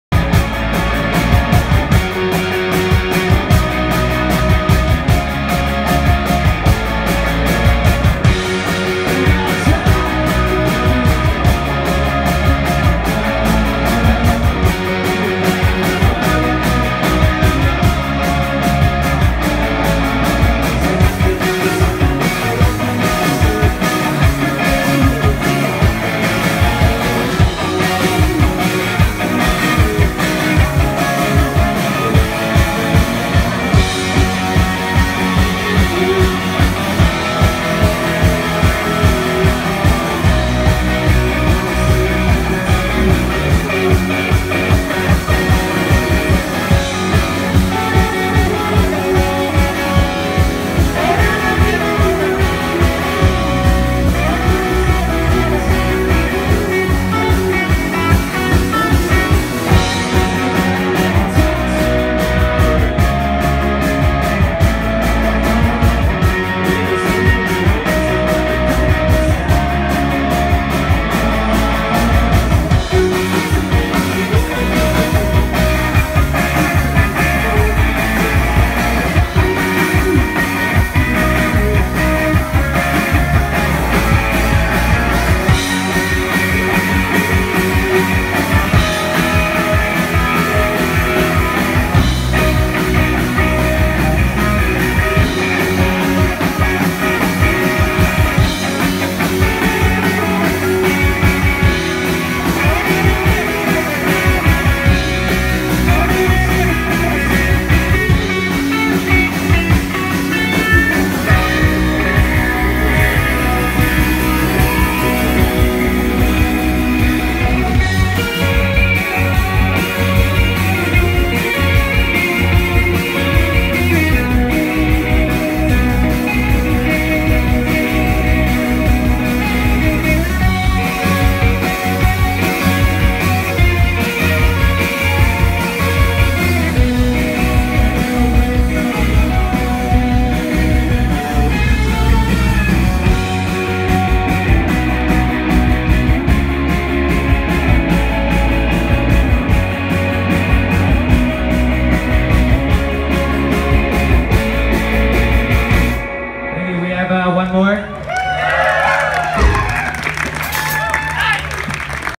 90's Television Salt Lake City, Utah Band Twilite Concert Series 7.27.2017
90's Television Salt Lake City, Utah Band Twilite Concert Series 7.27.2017. Local opening act for Whitney and Kurt Vile